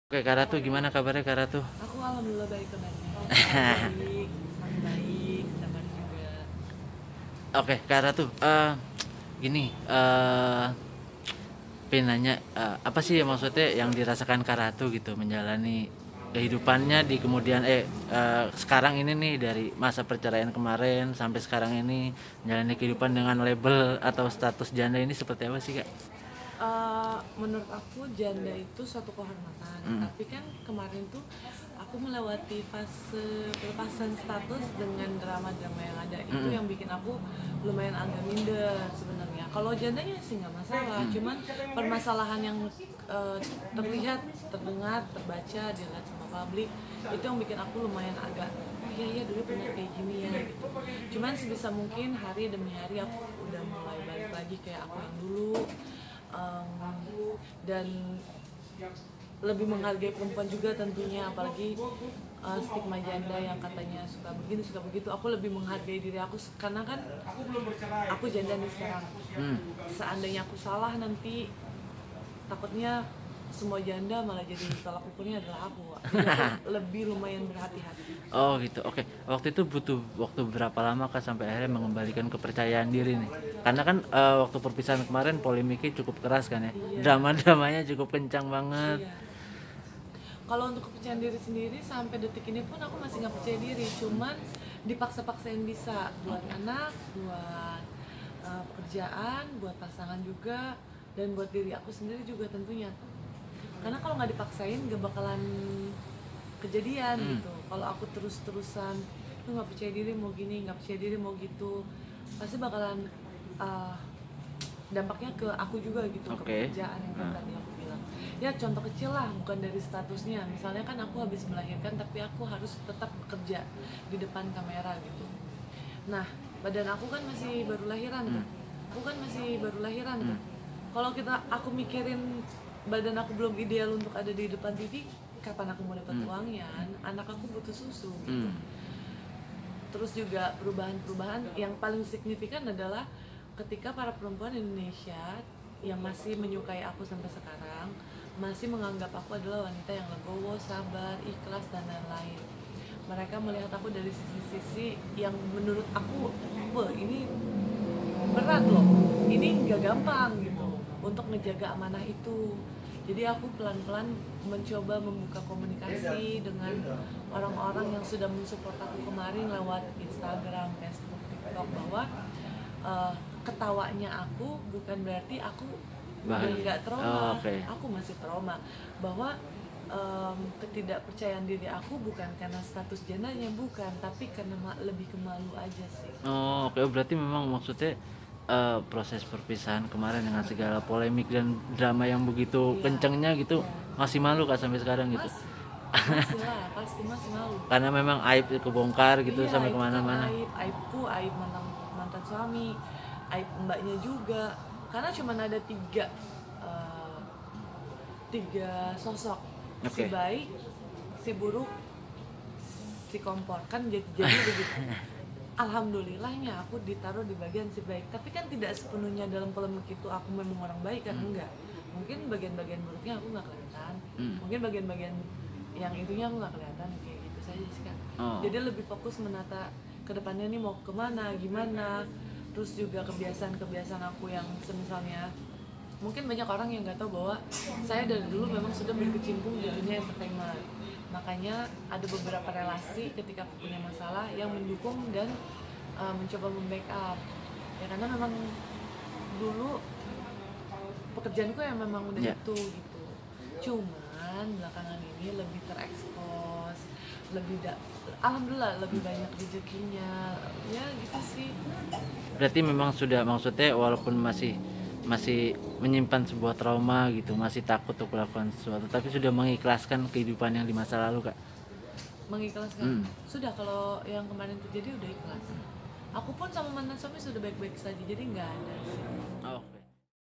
Perjuangan Ratu Rizky Nabila Jalani Hidup Menjanda
Selebritas Ratu Rizky Nabila menjadi sorotan tajam warganet, usai membongkar aib rumah tangganya dengan pesepak bola Alfath Fathier yang sudah bercersi sejak Mei 2021.

Pasalnya, semenjak dinikahi Alfath Fathier tahun 2020, Ratu Rizky Nabila mengaku menerima Kekerasan Dalam Rumah Tangga (KDRT), bahkan hal itu terjadi ketika dirinya hamil.

Tak sampai disitu, Alfath Fathier pun selingkuh dengan wanita bernama Nadia Christina.